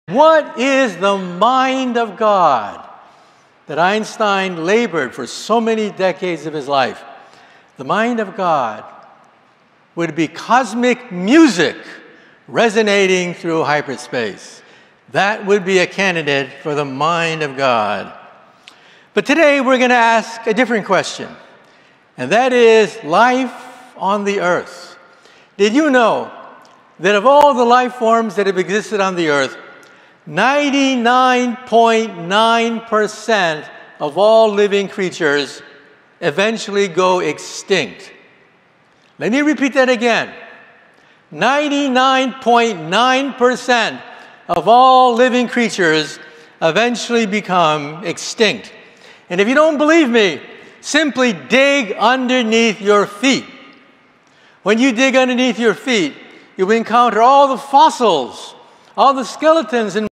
Michio Kaku - What Is The Mind of God?
Michio Kaku explains whats is the mind of god. ‘The mind of God we believe is cosmic music, the music of strings resonating through 11 dimensional hyperspace. That is the mind of God.”